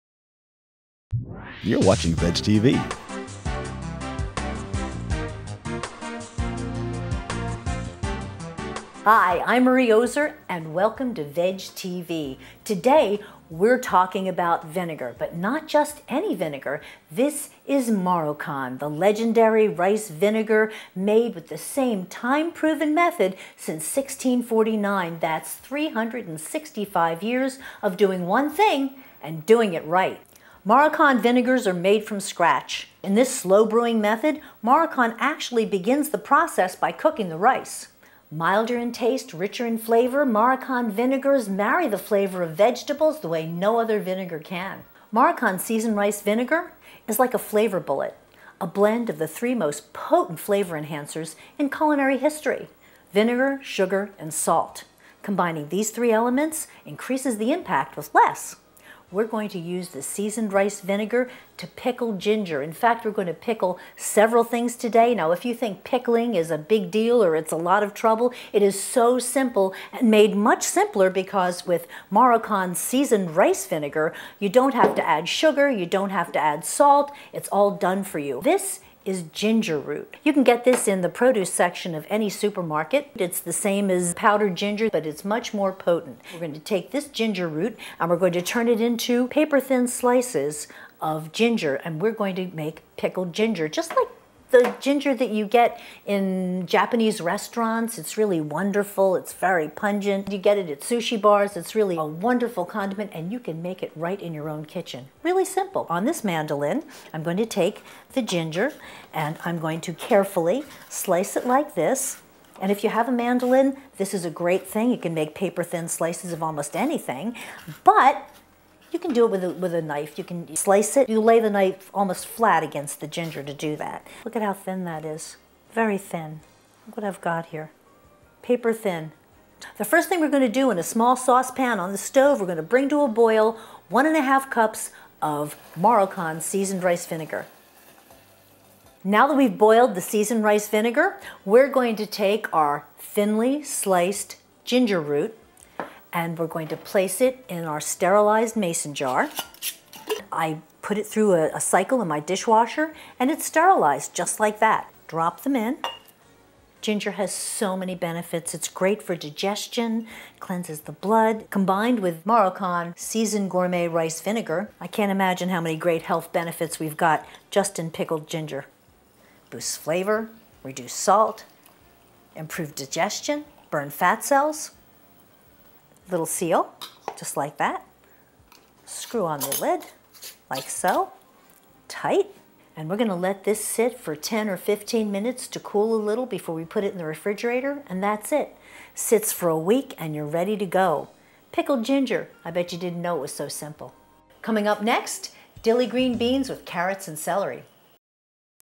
Marie Oser talks Marukan Vinegar/Pickled Ginger
How Marukan creates their rice vinegar and what it can be used for. No need for sugar or salt.
Creating Pickled Ginger: Boil Marukan Season Rice Vinegar, take thinly sliced Ginger Root in sterilized mason jar. Pour boiled vinegar, refrigerate for a week.